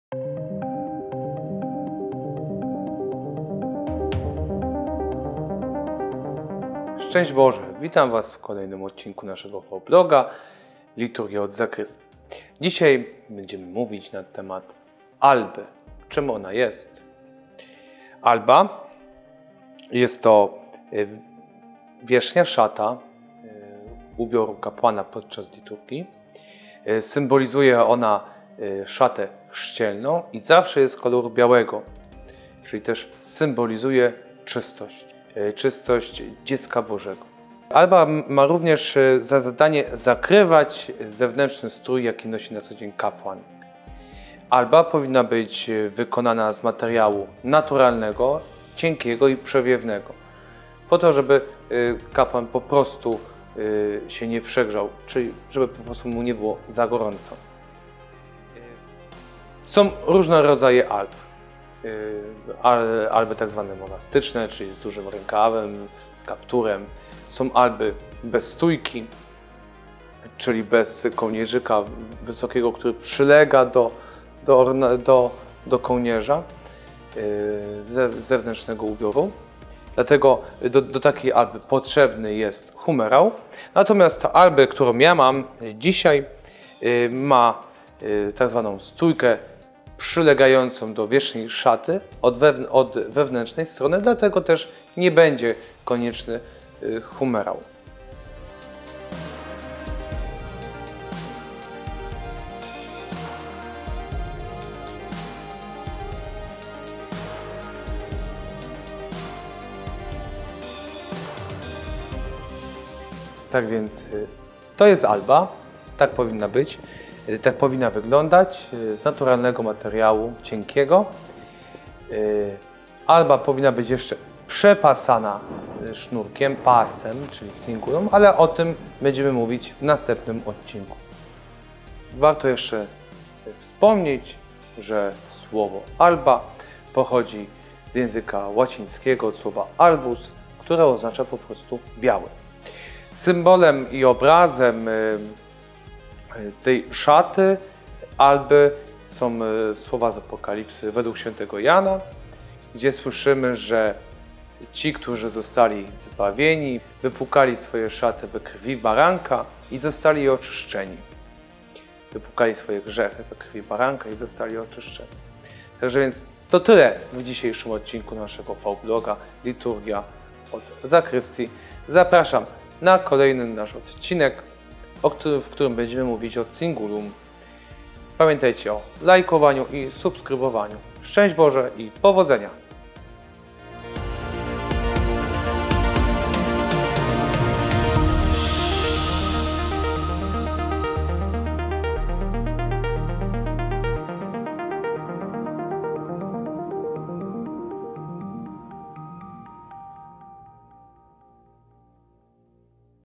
Liturgia od zakrystii [09]: Alba
Kolejny odcinek vloga ks. Tomasza Kotulskiego "Liturgia od zakrystii" poświęcony jest alba.